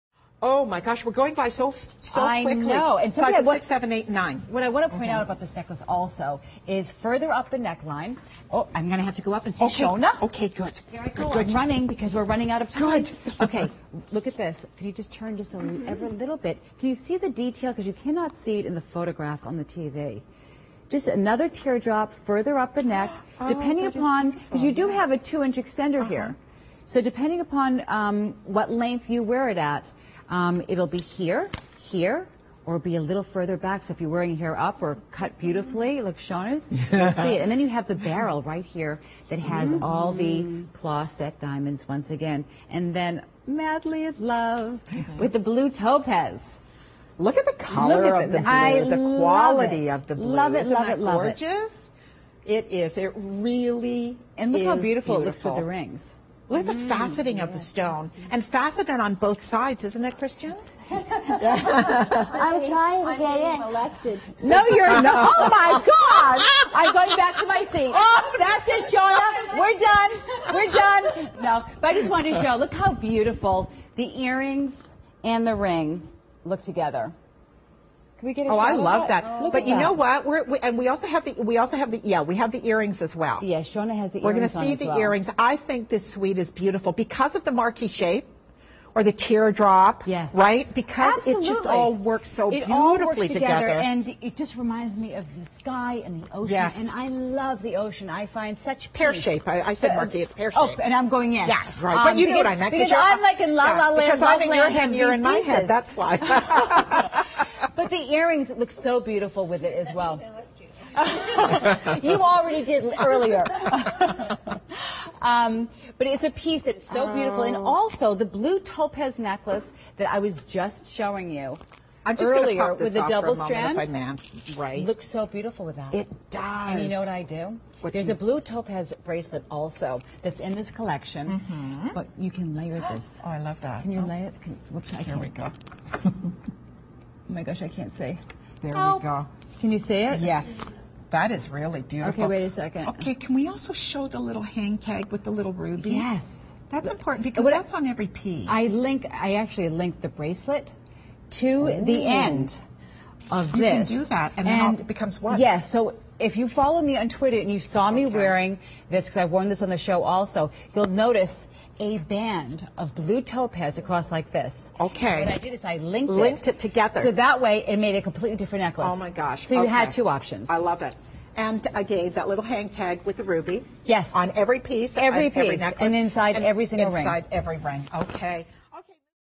Kristian Alfonso Sterling Silver Blue Topaz Lariat Necklace with Diamonds at The Shopping Channel...
Prices & promotional offers shown on this previously recorded video may no longer be available. Visit theShoppingChannel.com to view the current price.

You'll want to wear this breathtaking sterling silver blue topaz lariat necklace with diamonds from Kristian Alfonso at every opportunity. The Ancient Greeks wore topaz for strength. Always so striking in beauty, topaz is a symbol of love and affection, and Kristian's love of blue topaz inspired her to create this set. This delicate Blue Topaz Necklace is accented by one bezel-set tear drop on the chain.

• Sterling silver
• (16) round shaped diamonds (0.10 ctw), prong set
• (2) 15x8mm pear shaped blue topaz (10.00 ctw), bezel set
• (1) 7x5mm pear shaped blue topaz (0.90 ctw), bezel set
• (1) gypsy red ruby on the "Believe" logo
• Average gram weight is 7.50
• The necklace measures approximately 16" in length with a 2" extender
• The necklace has a 3.5" drop
• The necklace has a lobster clasp
• Diamond clarity: I2-I3; colour: G-H
• Nickel free